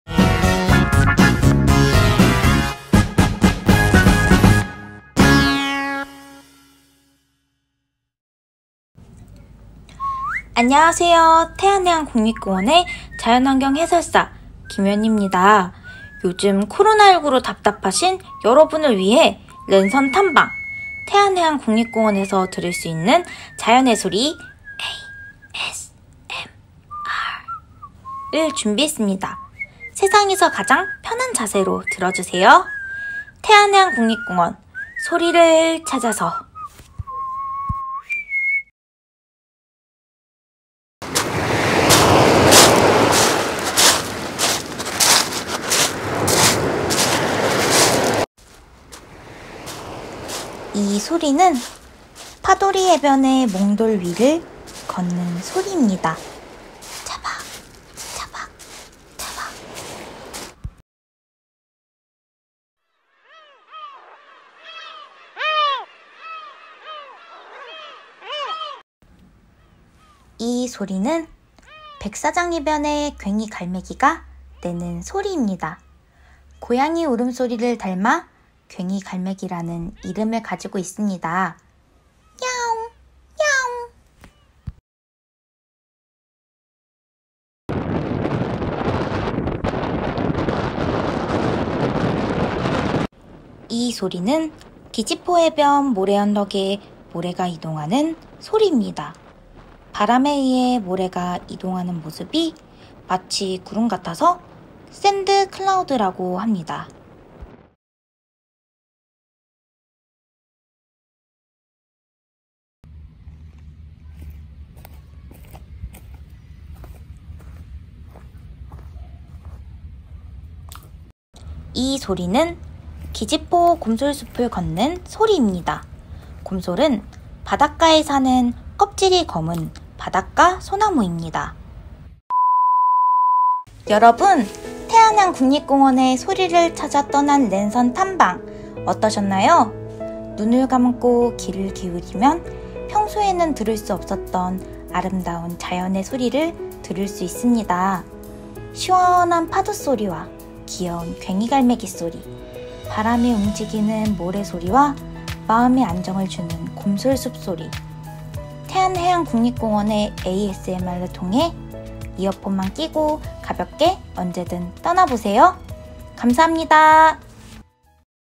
[UHD][태안해안국립공원] 태안해안국립공원의 소리를 찾아서
태안해안국립공원에서 태안해안의 자연의 소리를 들려드립니다.